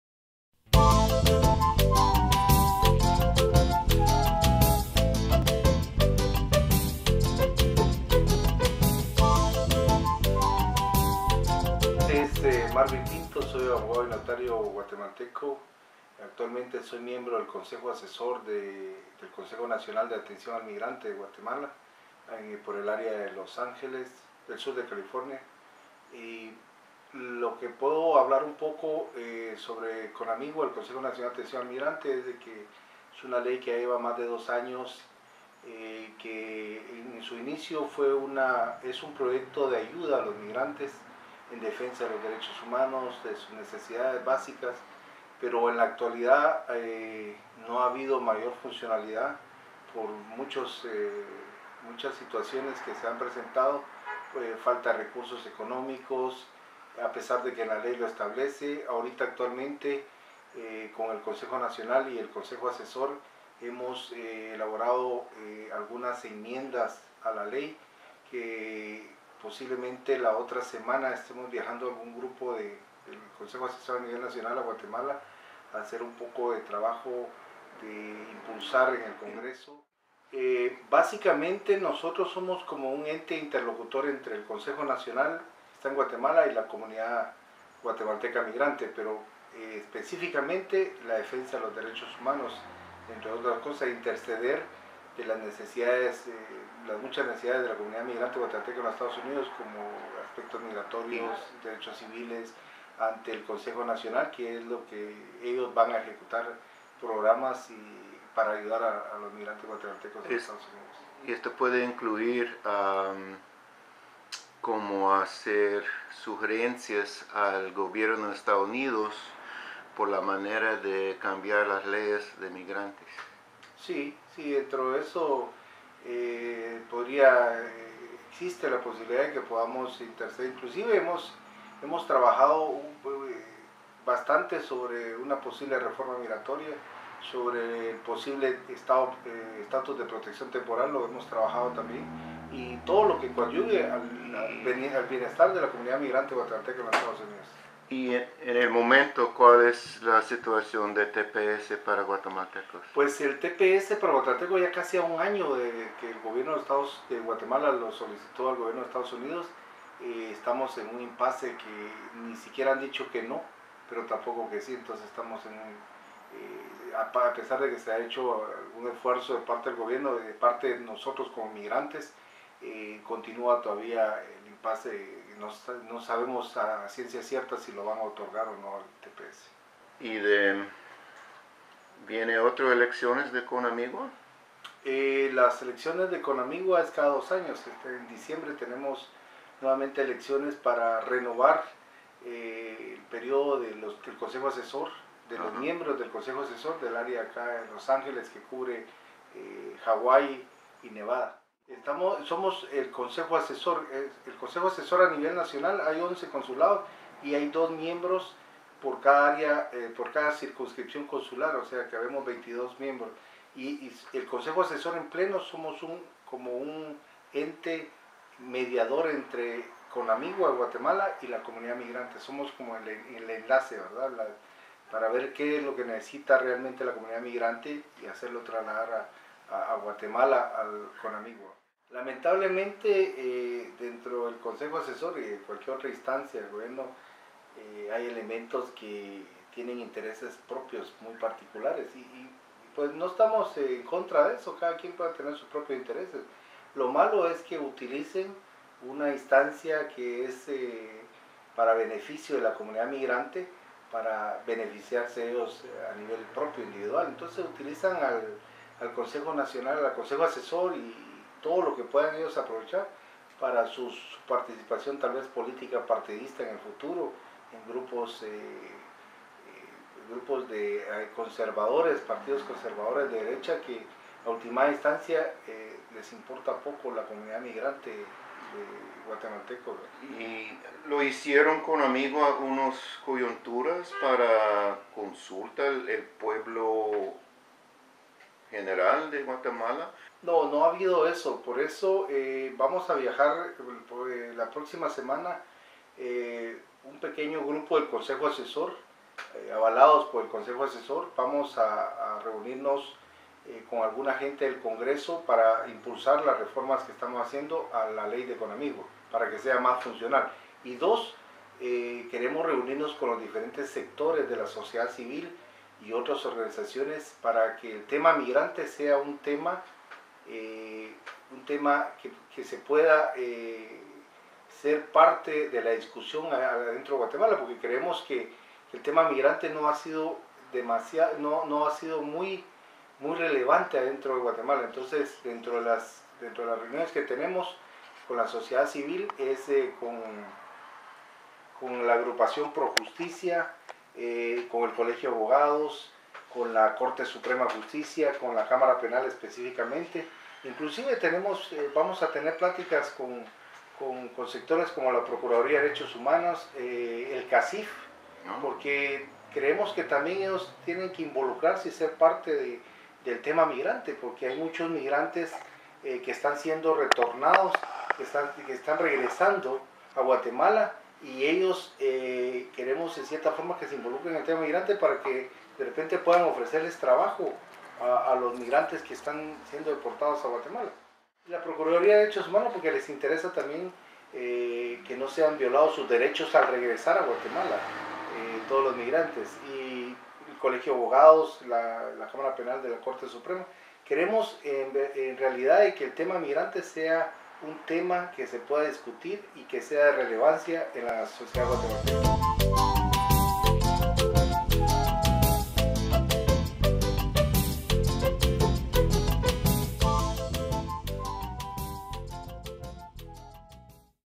Marvin_0610_11_mp2.mpg
Interview with National Advisory Council for Guatemalan Immigrants CONAMIGUA member, Lic. Marvin Pinto in Spanish with English subtitles. Advisory Council member speaks on the agency charged with protecting the Rights of the Guatemalan immigrant community in the United States and promoting their welfare.